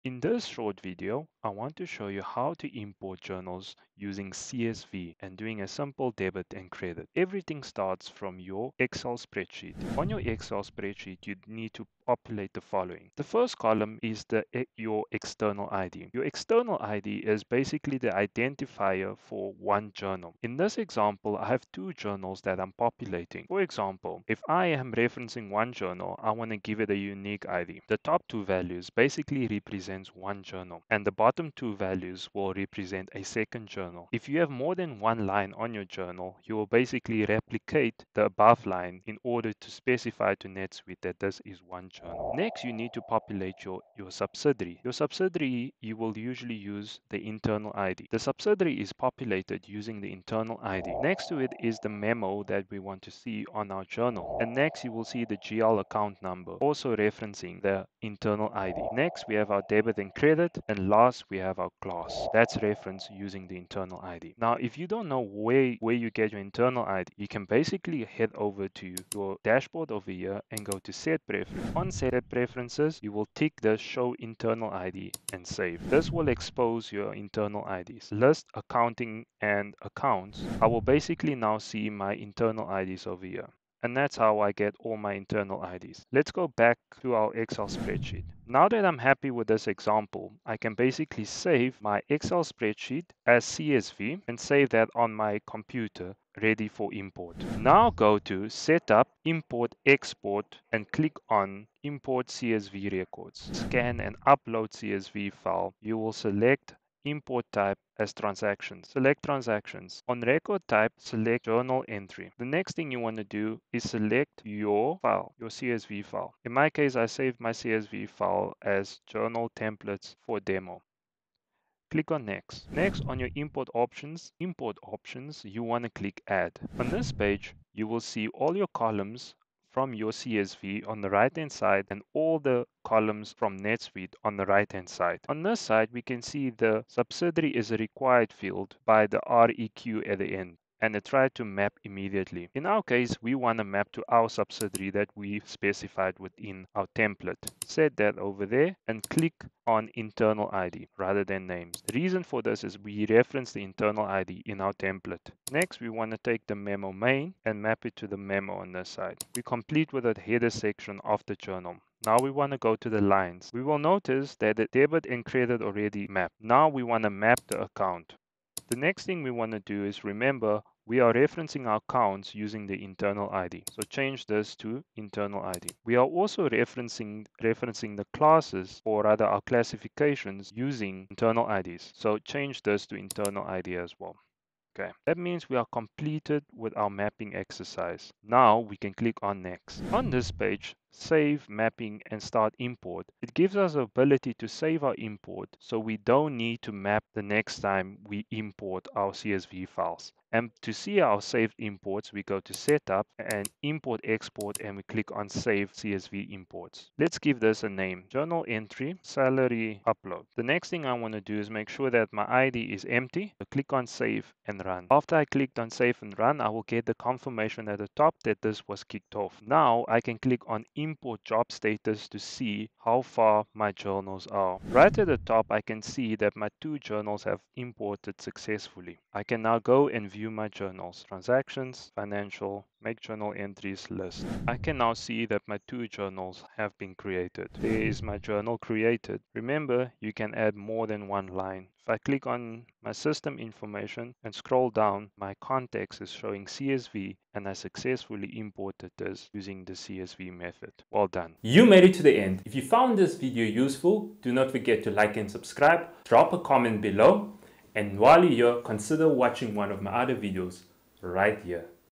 Upload Journals To Netsuite Using CSV
Tired of tedious month-end journal entries?

Ditch the manual work and bulk upload your journals in NetSuite with CSV!

This step-by-step tutorial shows you exactly how to:

Save hours on your month-end closing process.
Upload salary journals, expense reports, and more, in minutes.
Eliminate errors with accurate CSV formatting.
Master the NetSuite Import Assistant for stress-free uploads.
No more late nights spent entering data!  This method is perfect for accountants, bookkeepers, and anyone who wants to streamline their workflows.

In this video, you'll learn:

The specific CSV template needed for journal entries.
How to map your data fields to NetSuite's format.
Common pitfalls to avoid and ensure a smooth import.
Bonus tips for optimizing your bulk uploads.
By the end of this tutorial, you'll be a CSV importing pro!